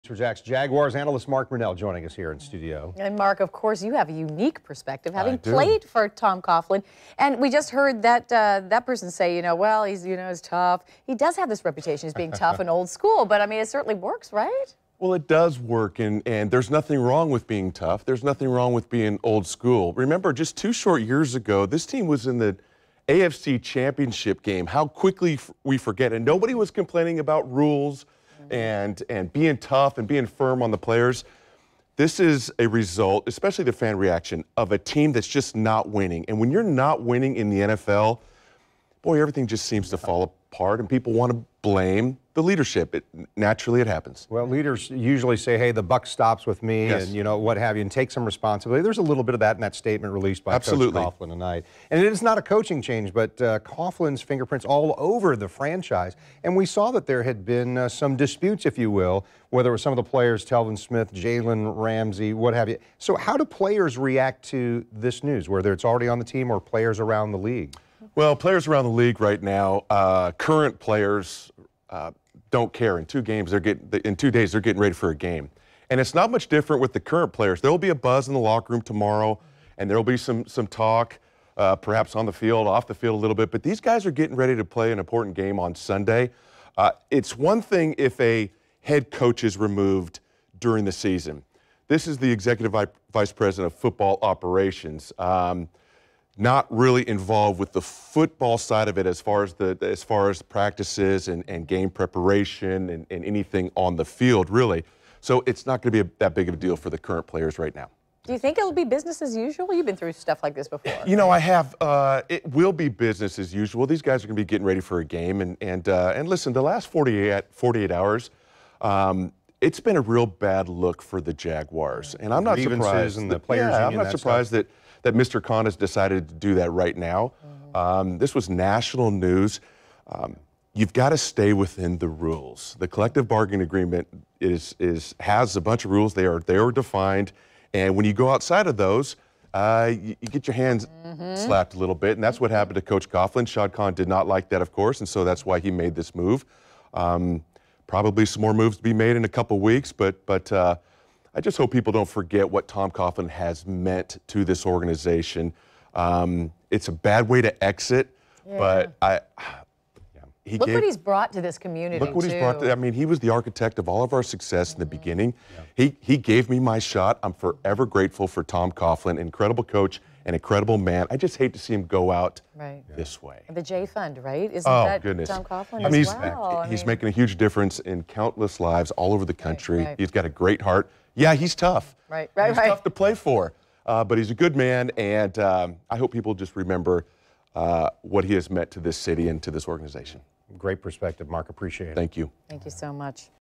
News4Jax Jaguars Analyst Mark Brunell joins us in the studio
We hear his take on Tom Coughlin getting fired